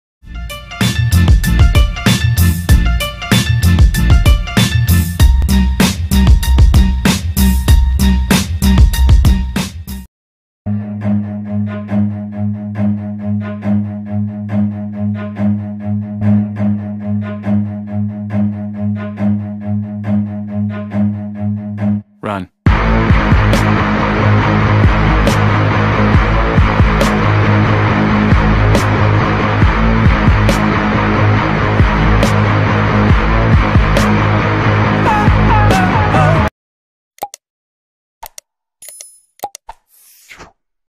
Intro Fight Corona Virus
Lets fight the virus by simple steps, Wear Mask + Social Distancing + Hand Washing... Stay Safe EveryOne...